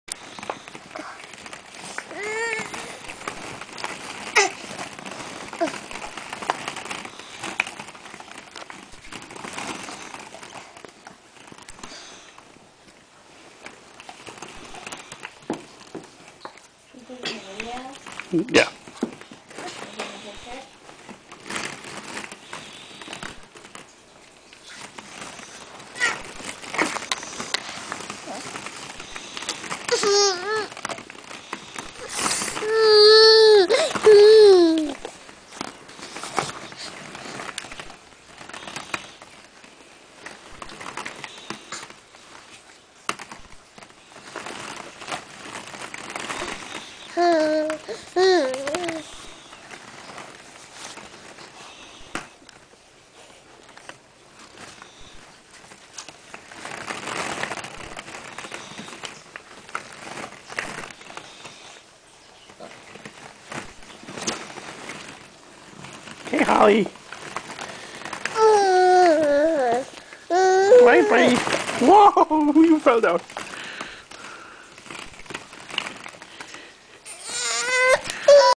Ball Pit
Holly Hanging in the Ball Pit trying to stack Balls on top of her head.